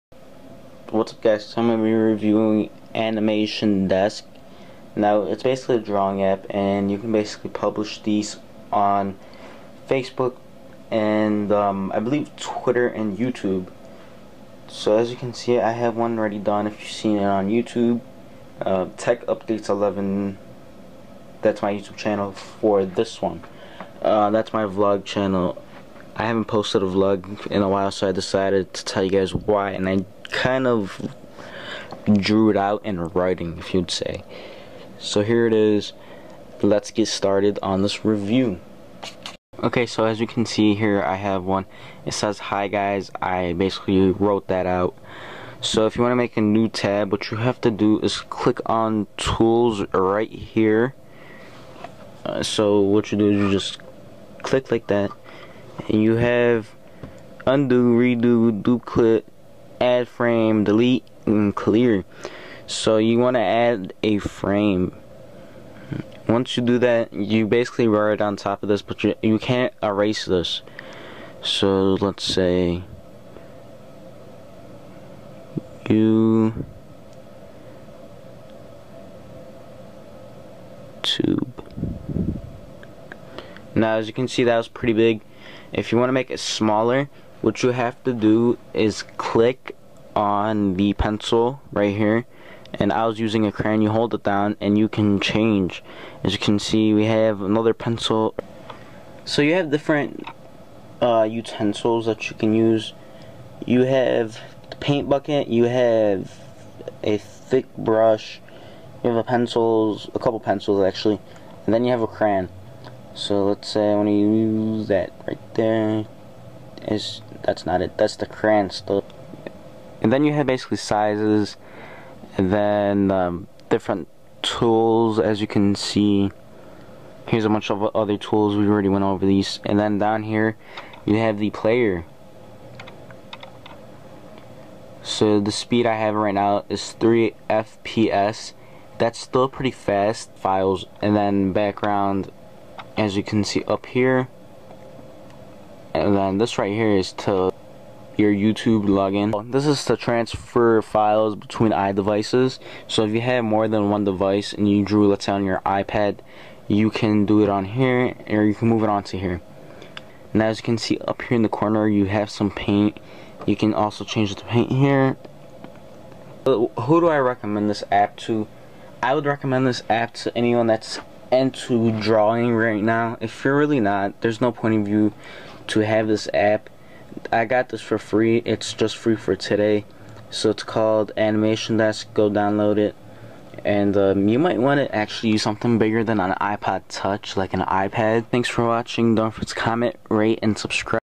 Animation Desk for iPad/ipod/iphone
i reviewed animate desk its just a drawing tab i would recommend that you use this on an ipad or when the new iphone comes out its a pretty good app has colors to draw different objects and to copy you can make videos and post it on youtube the app is free today so go download it NOW! here is the main features:
Main Features:

✔Friendly and gorgeous user interface
Realistic scene for creating animation

✔Share your masterpiece with the world
Upload your animations directly to YouTube and Facebook

✔Send animated E-cards
Send your animated work as email attachment to wow your friends

✔Layers
Foreground and Middle Ground supported

✔Dynamic and static background images
Animate your ideas with ease by applying a dynamic background, or insert your favorite picture as the static background

✔Audio support
Bring your animated creation to life by adding background music or sound effects

✔Stamp tool
Use our patented stamp features to add shapes, insert pictures, or copy and paste selected area on a frame

✔Easy file management
Create and manage files under the thumbnail mode

✔Powerful frame manager
Editing, coping, moving, deleting frames is like a summer breeze

✔Save to PDF
Export your animations to PDFs

✔Various painting tools with pressure sensitivity
Bucket fill tool, pencil, crayon, three types of brush and eraser

✔Adjustable size and opacity for the brushes and eraser
Manage every stroke to your liking

✔Onion Skinning
See an overlay of adjacent frames

✔Color selection palettes and swatch panels
Decorate your creative ideas with more than 100 vivid colors

✔Palm rejection feature
Rest your hand on the screen without activating other functions

✔Anizone
 An Exclusive YouTube Platform for Animation Desk Users to watch, share, and give feedback on animation works created by other Animation Desk users sit back and watch the video.